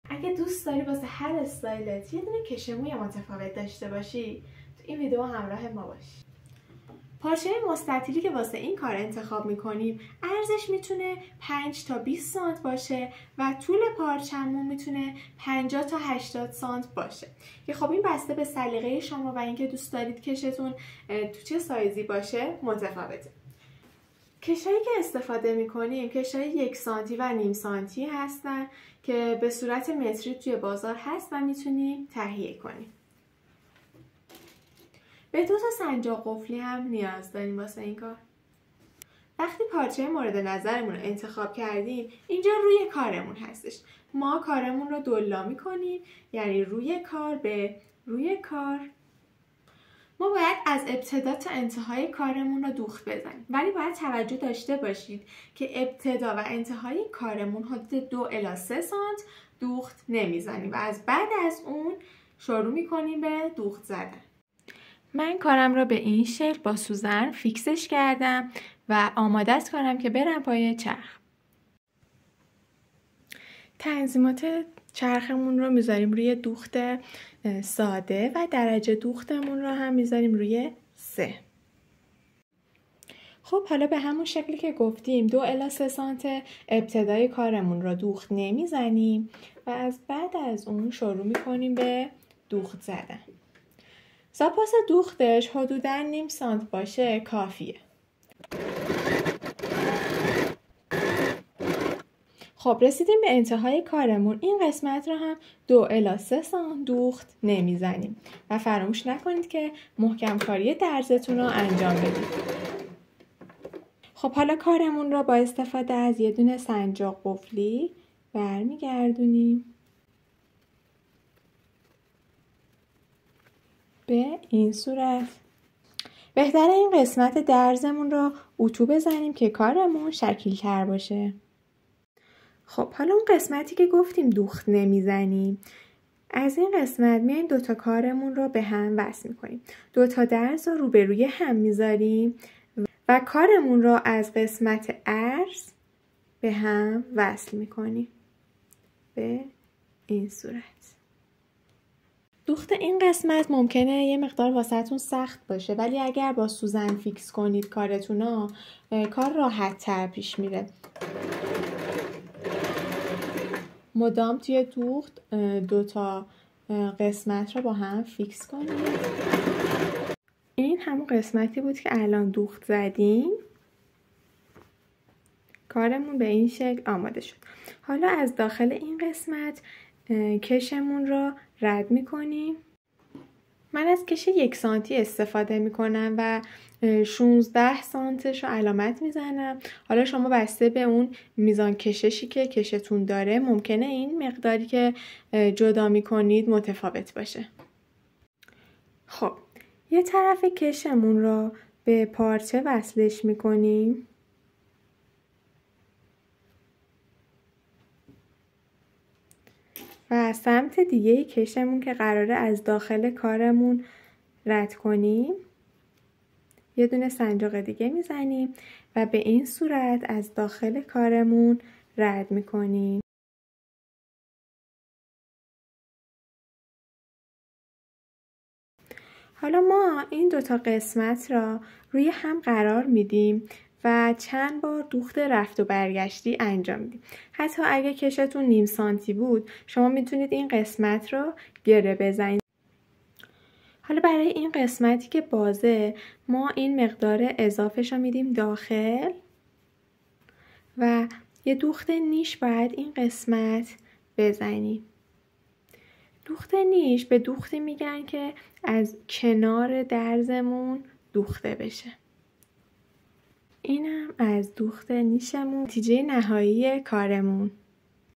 آموزش کش مو با یه روش ساده، واسه هر استایلت یه کش مو بساز (کش موهاتو رایگان خودت بساز)کش_مو
هنوزم کش مو میخررری؟؟؟
با دیدن آموزش کش مو به راحتی میتونی واسه هر استایلت یه کش مو شیک بسازی و خفن ترین اکسسوری هارو با کم ترین هزینه درست کنی